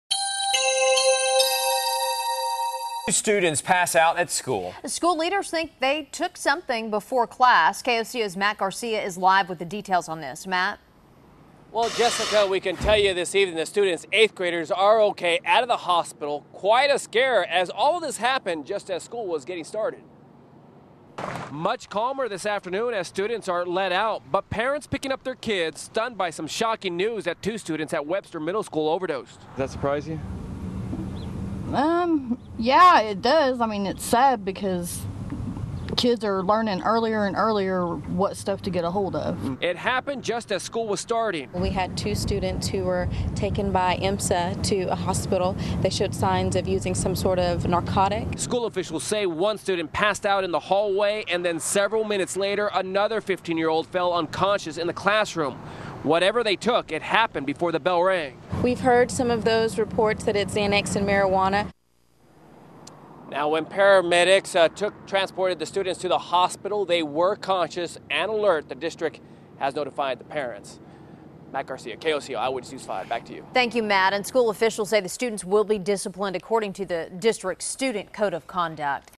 Students Pass Out At OKC Middle School
School officials investigate an incident in which two students pass out at Webster Middle School -- and officials think a substance could be to blame.